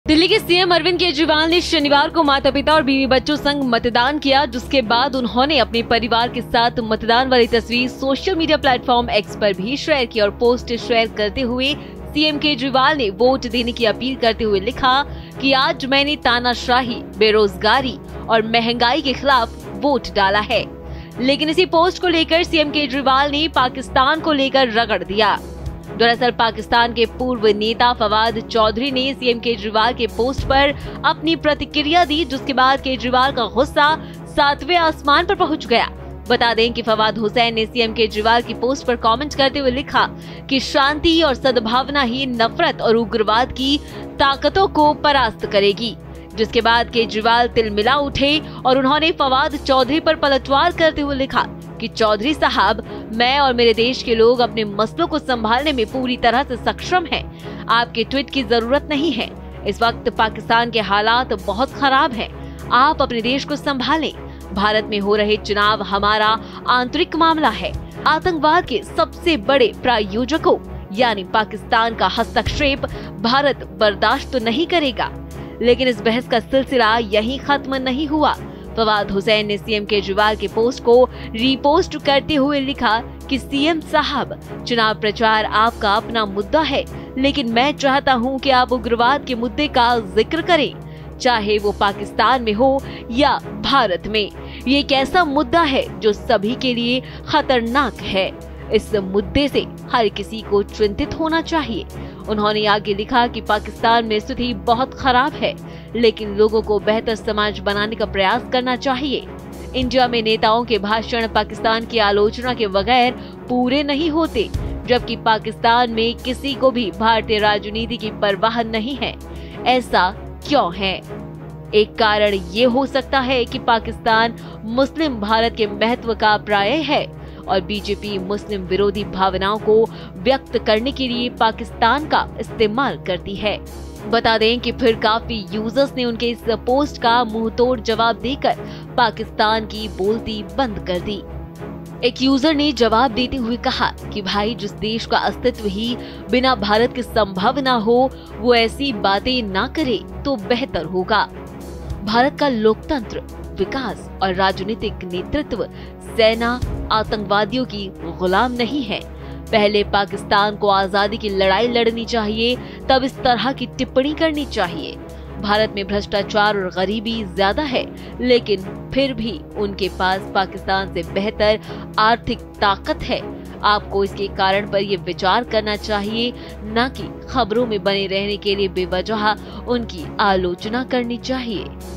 Arvind Kejriwal On Fawad Chaudhary: Pakistan के नेता को केजरीवाल ने दिया करारा जवाब
दिल्ली के CM अरविंद केजरीवाल ने शनिवार को माता-पिता और बीवी-बच्चों संग मतदान किया. जिसके बाद उन्होंने अपने परिवार के साथ मतदान वाली तस्वीर सोशल मीडिया प्लेटफॉर्म X पर शेयर की और पोस्ट शेयर करते हुए सीएम केजरीवाल ने वोट देने की अपील करते हुए लिखा कि आज मैंने तानाशाही, बेरोजगारी और महंगाई के खिलाफ वोट डाला है...
Producer -  Himanshi
Editor- Harvinder Singh

Thanks & Regards
Bharat Express